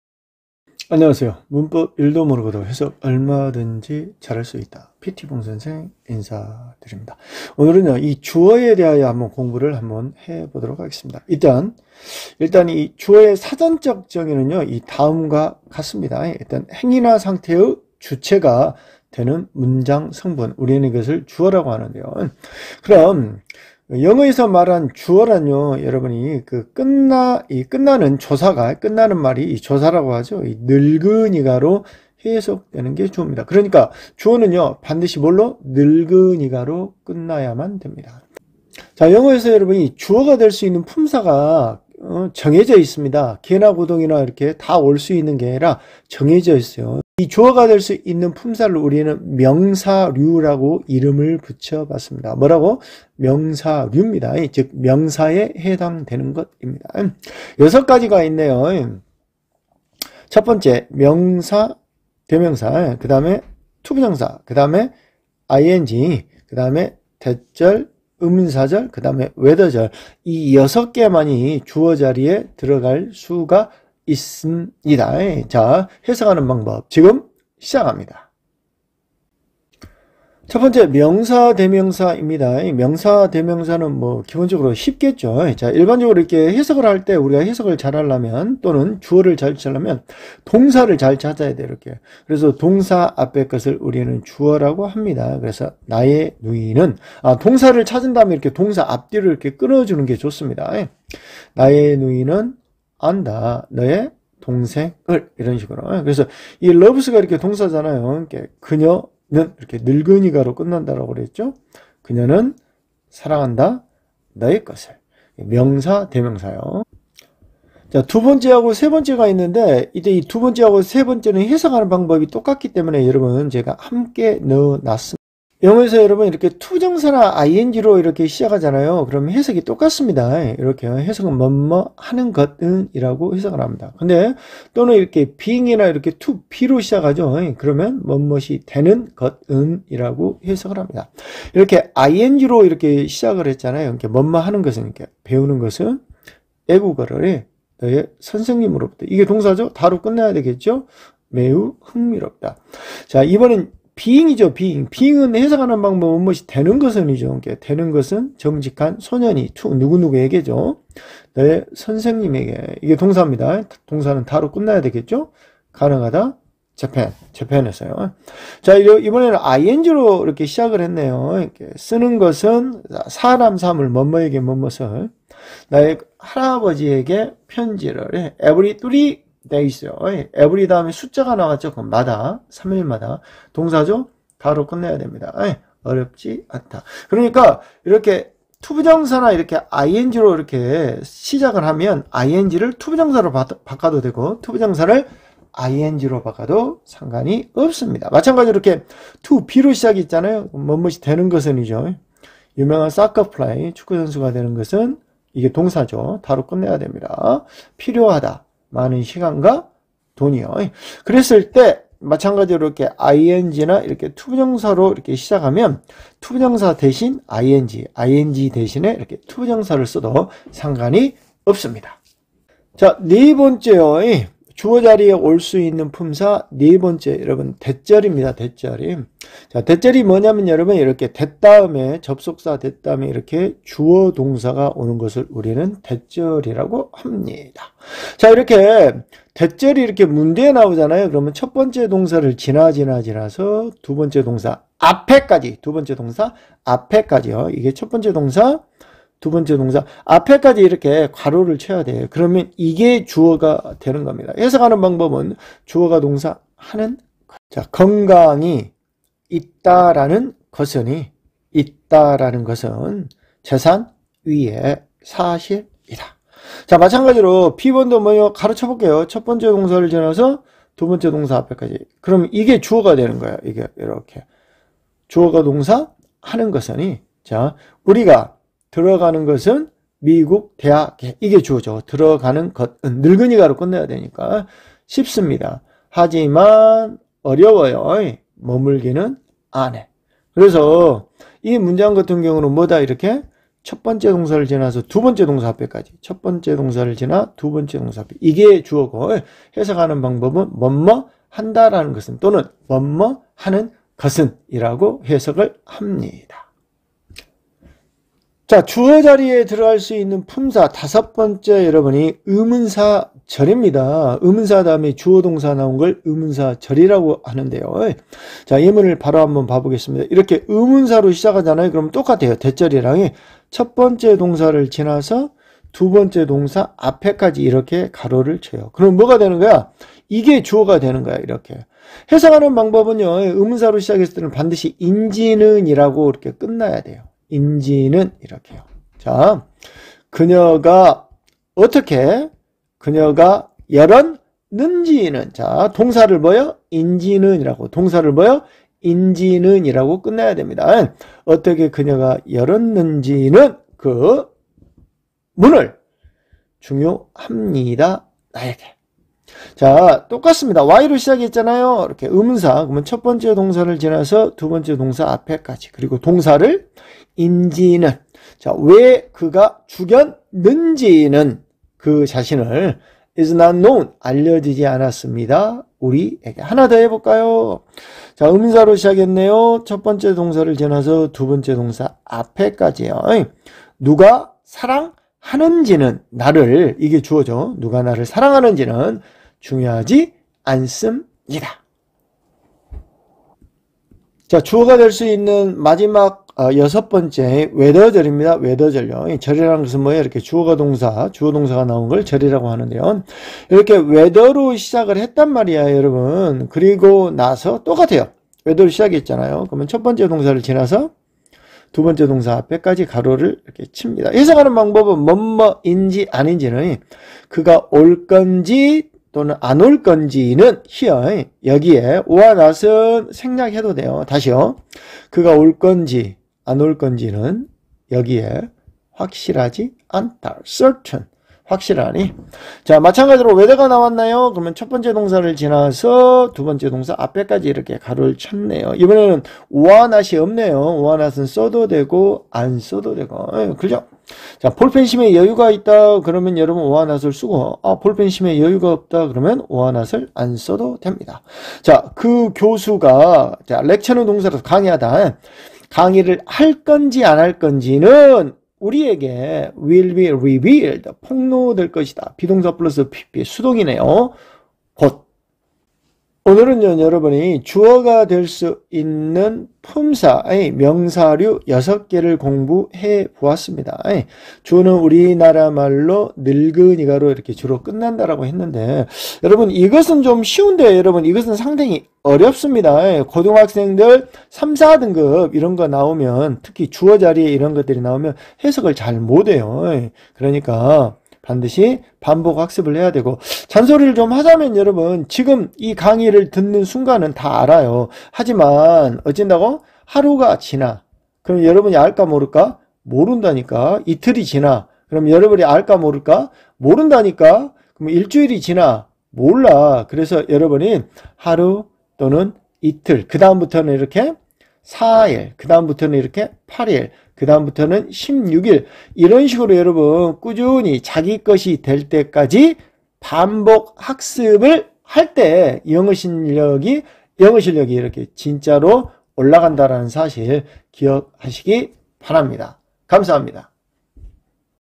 주어만 찾아도 문장이 쉬워집니다.
주어가 들어갈수 있는 명사류 6개와 해석방법